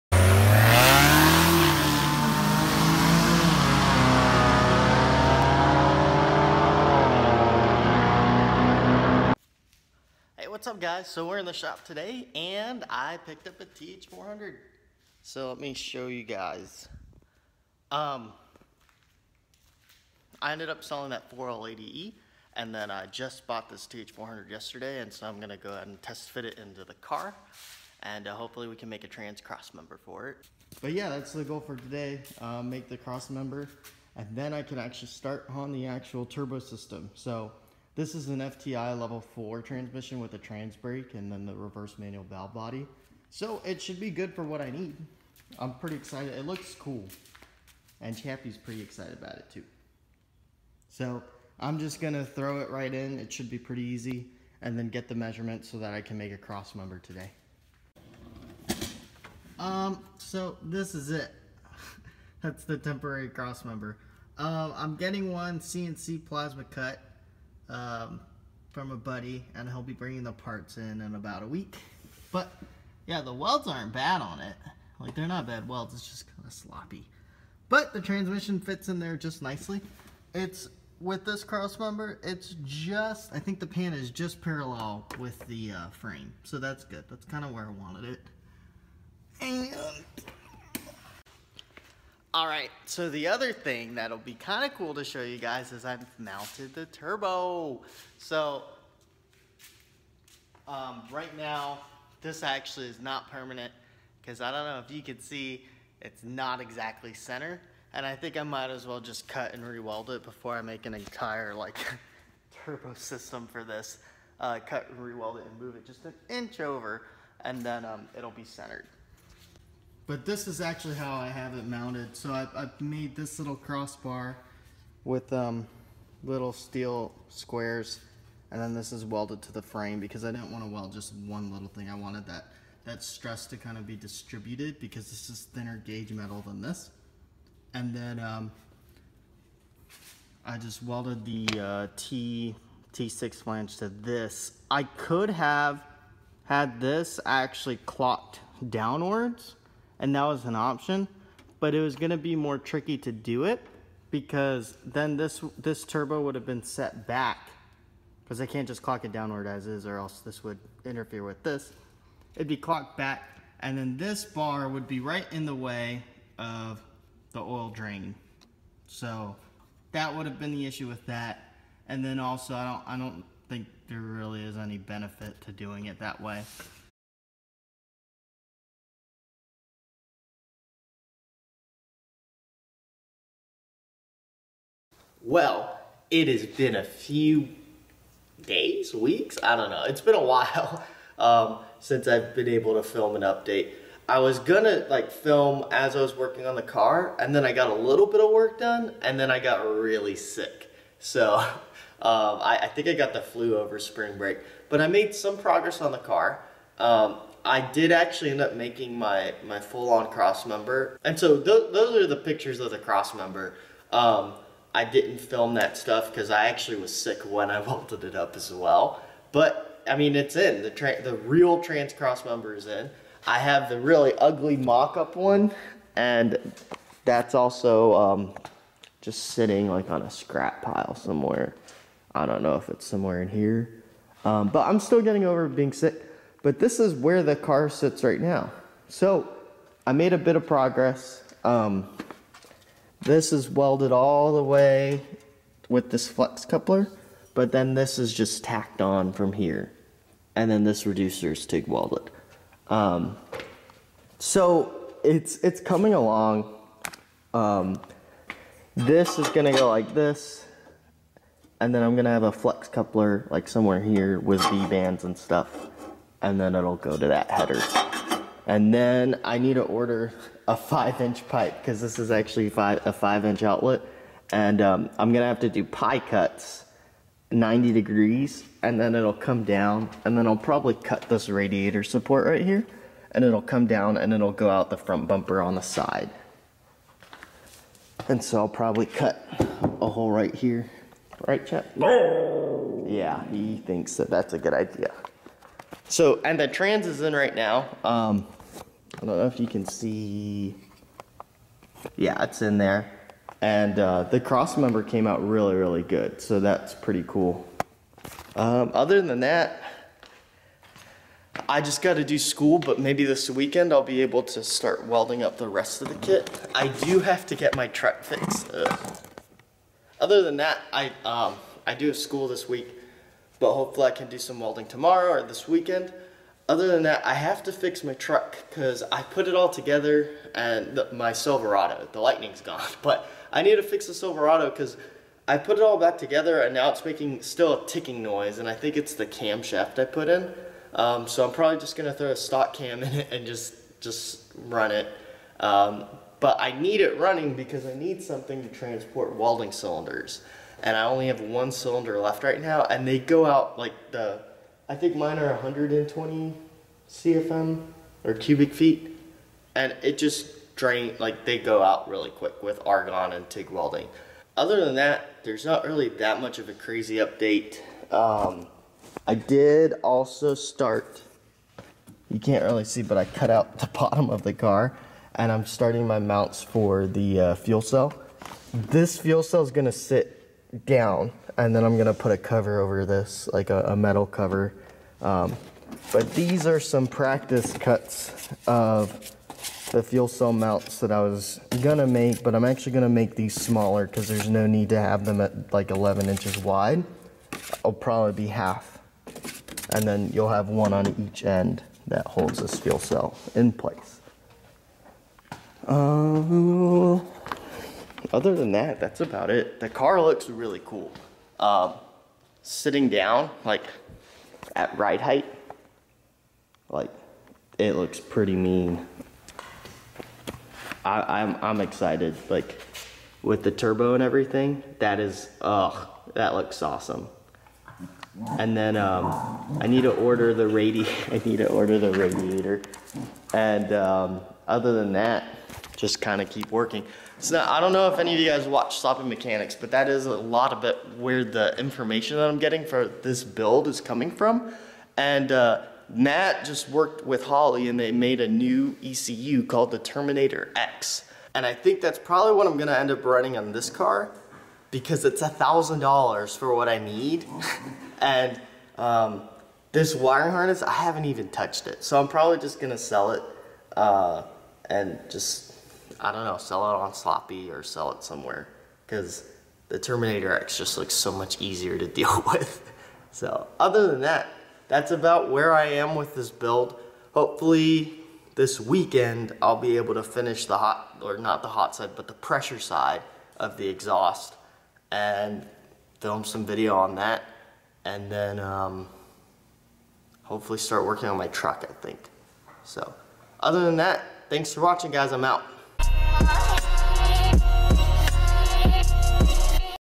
Making My Ls Turbo Kit
Got a Th400 since the 4l80 was not going to fit. Started on fabricating the hot side for my LS swap.